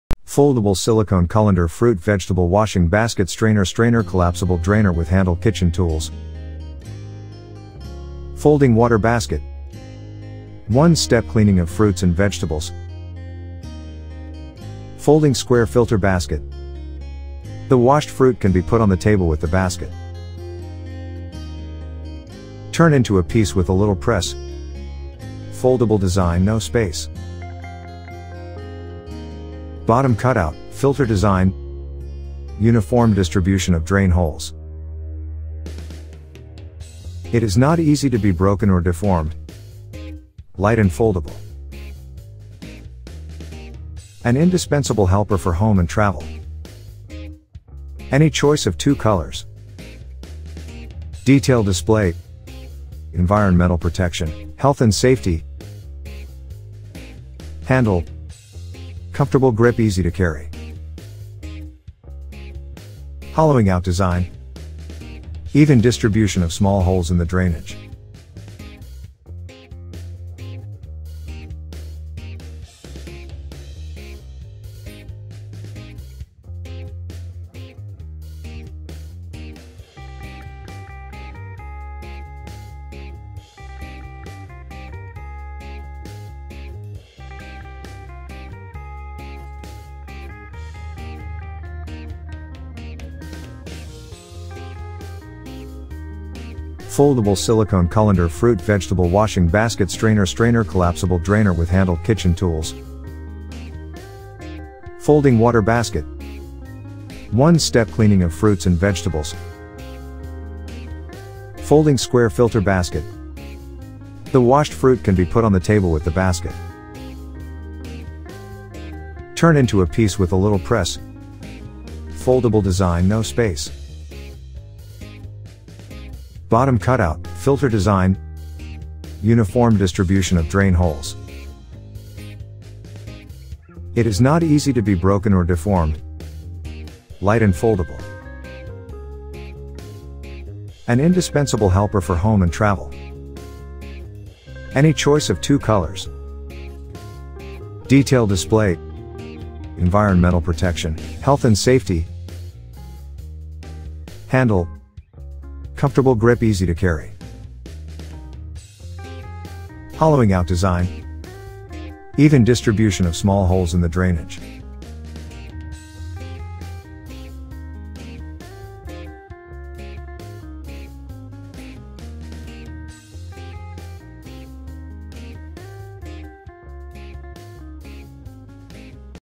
Foldable Silicone Colander Fruit Vegetable Washing Basket Strainer Strainer Collapsible Drainer
Foldable Silicone Colander Fruit Vegetable Washing Basket Strainer Strainer Collapsible Drainer With Handle Kitchen Tools

Folding water basket : One step cleaning of fruits and vegetables
Folding square filter basket : The washed fruit can be put on the table with the basket
Turn into a piece with a little press : Foldable design, no space
Bottom cutout, filter design : Uniform distribution of drain holes
It is not easy to be broken or deformed : Light and foldable
An indispensable helper for home and travel
Any choice of two colors
Detail display : Environmental protection, health and safety
Handle : Comfortable grip Easy to carry
Hollowing out design : Even distribution of small holes in the drainage

Foldable Silicone Colander Fruit Vegetable Washing Basket Strainer Strainer Collapsible Drainer Review by iFast Review